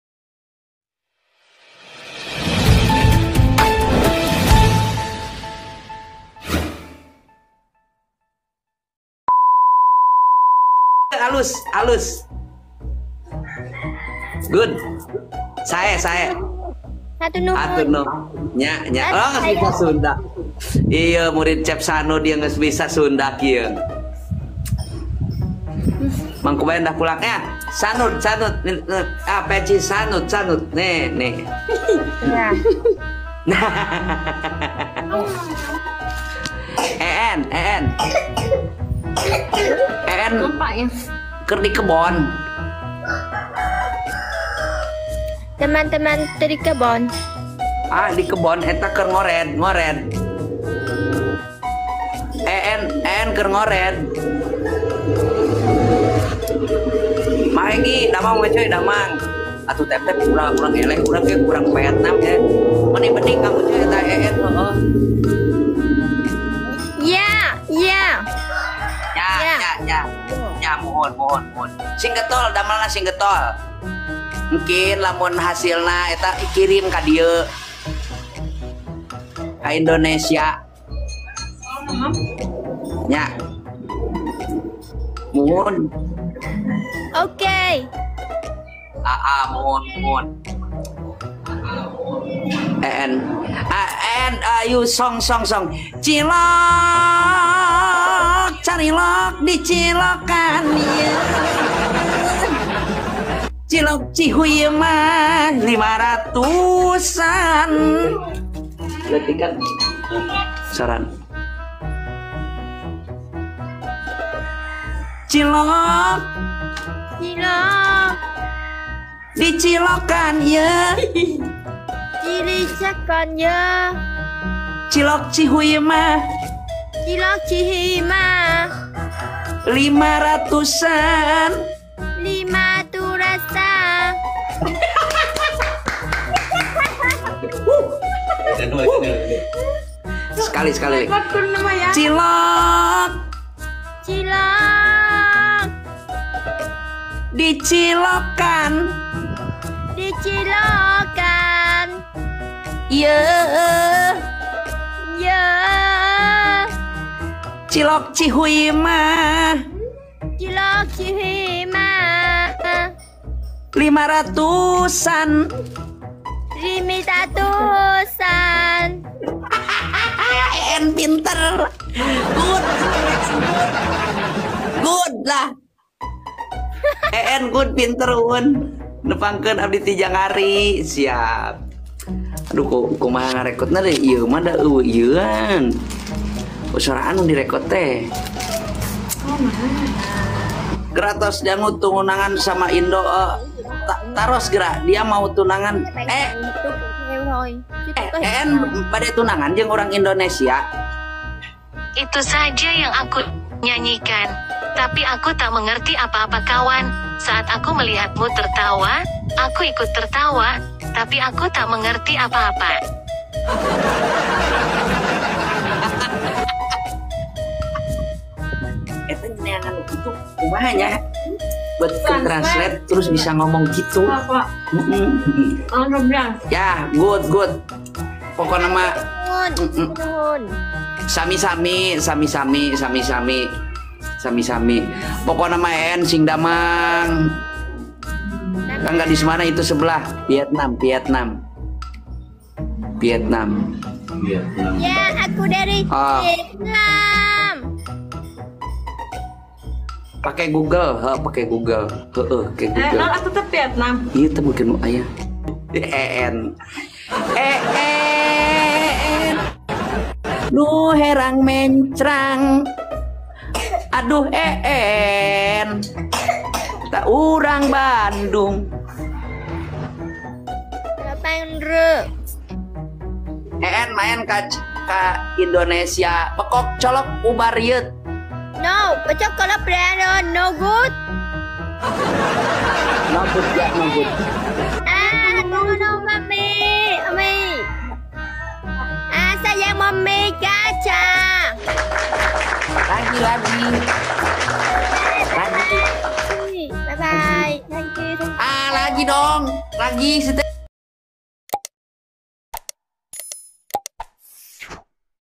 QUEENNY MELONGO KEBINGUNGAN Saat Diajak Ngobrol Pake Bahasa Sunda Sama Ucuy
een kebingungan saat diajak ngobrol sama tiktoker ucuy.